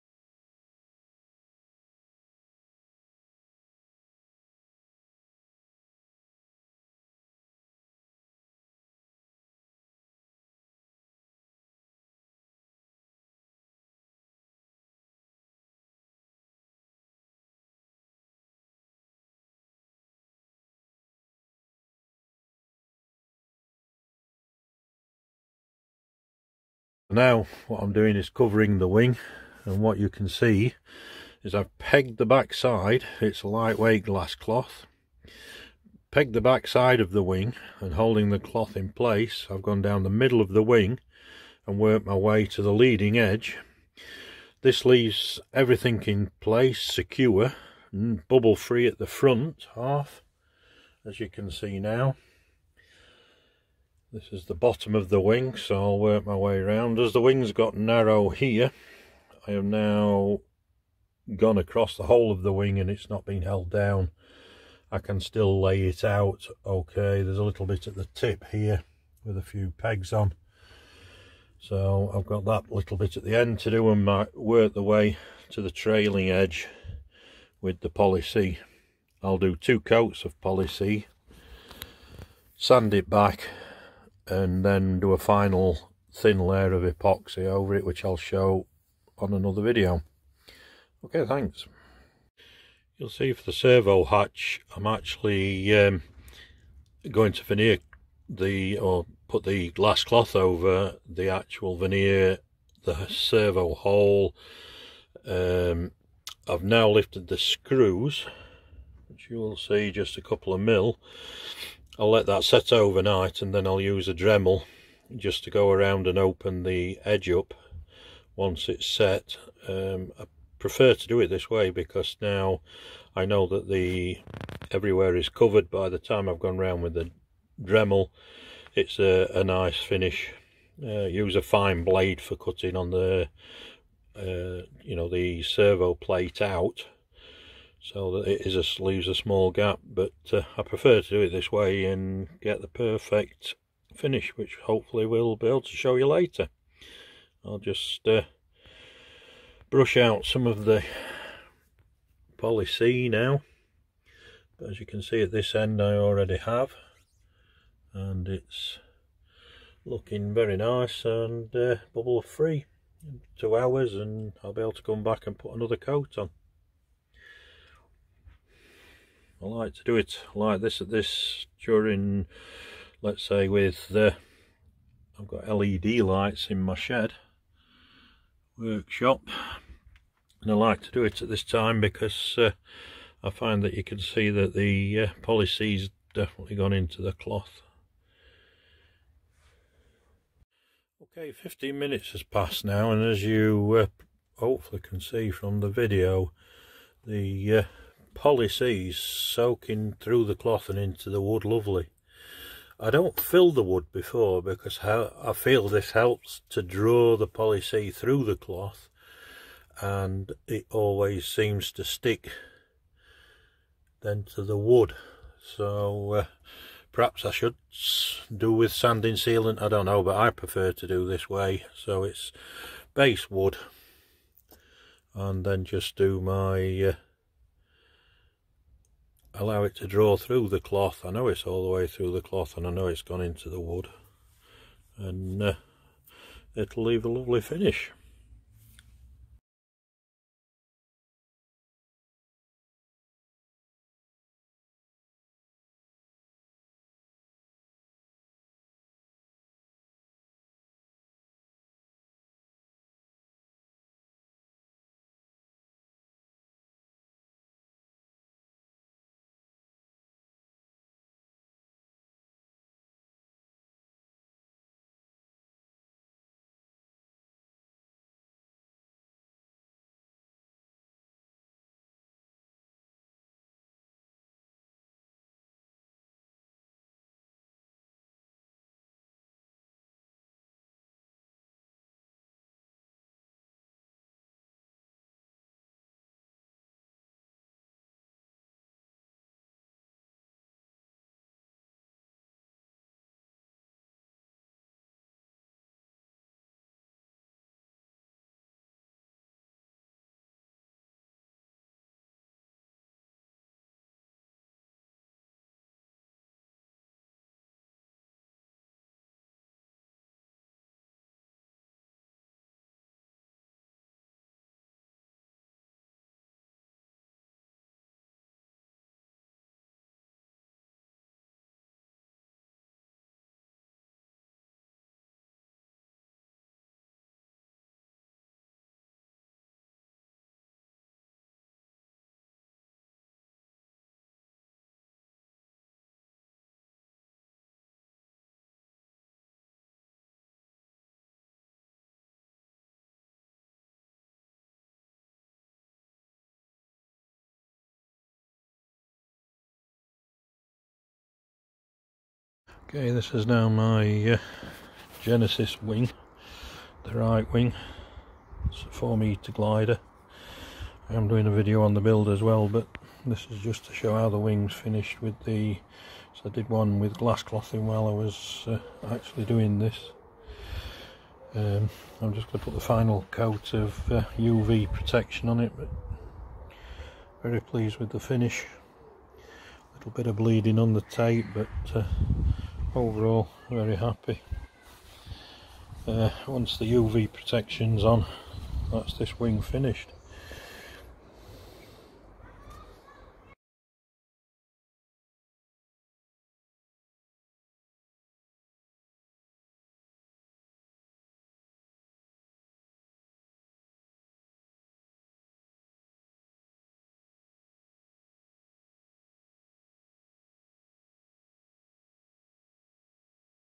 Glass Clothing an RC Sailplane wing
This is a video showing how I apply glass cloth to the wing of an RC Sailplane. My method shows how I use Poly C to get the glass cloth laid bubble free and without pulling the glass fibers before adding a layer of epoxy.